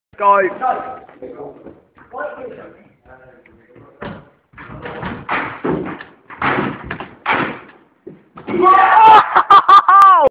4 Man ALLEY-OOP
FAIRLANDS YOUTH FC
DanaCup 2012
Hirsthals School
(Ryan Ballard, Joe Gerrard, David Spicer and Daniel Keegan)